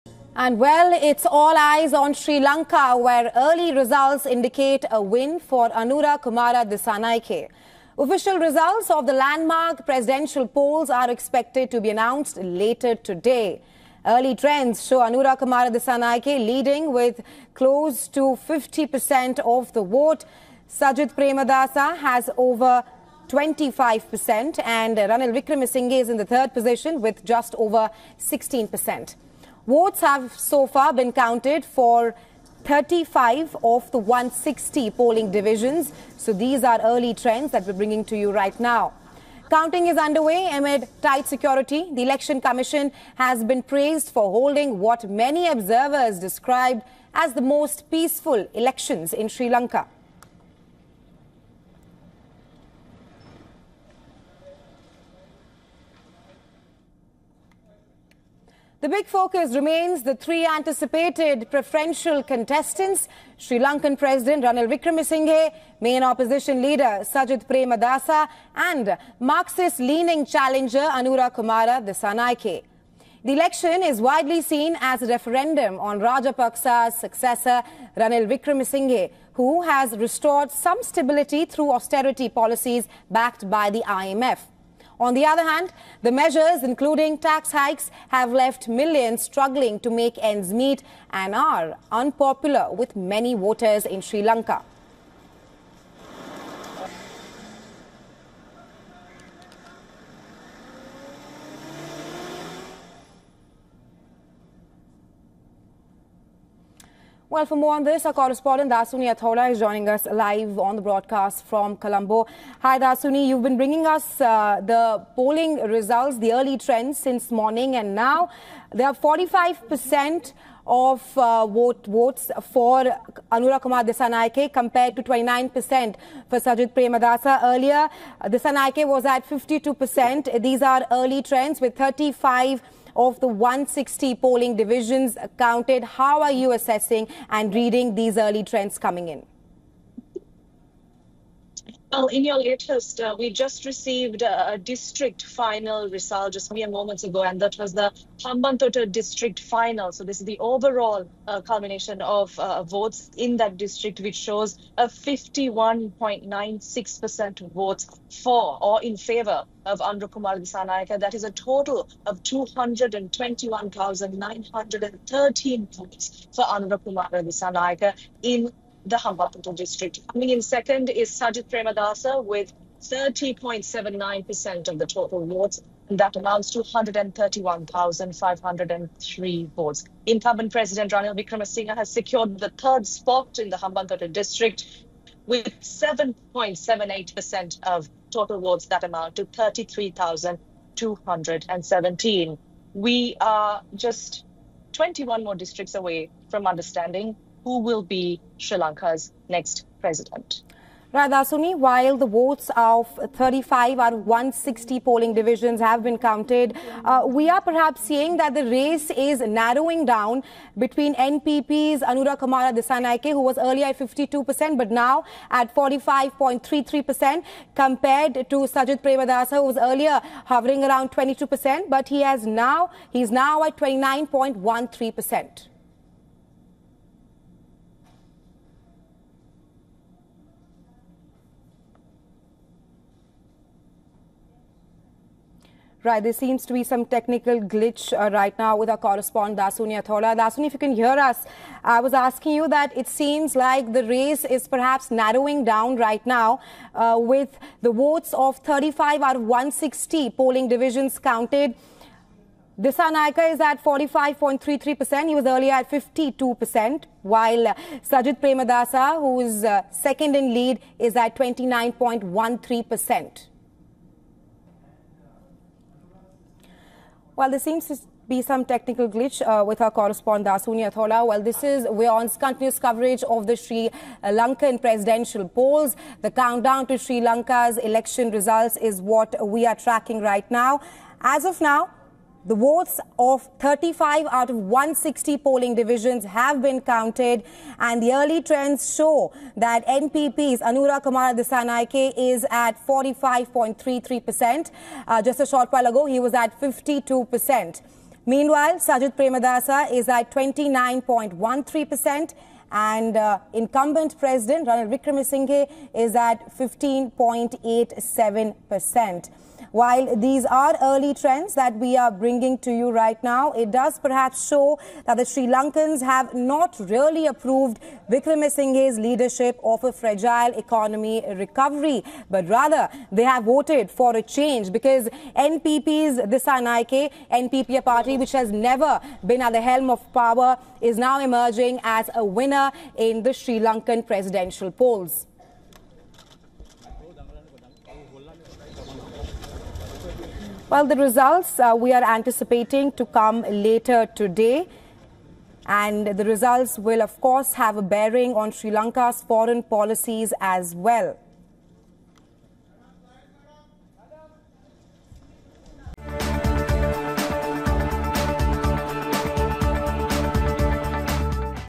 Sri Lanka Presidential Election: Counting of votes underway, what are the early trends? | WION
Polls closed in Sri Lanka's first election since the economic collapse, and the counting of votes is underway. Millions of voters cast their votes to select a leader who will face the task of bolstering the country's fragile economic recovery following its worst financial crisis in decades.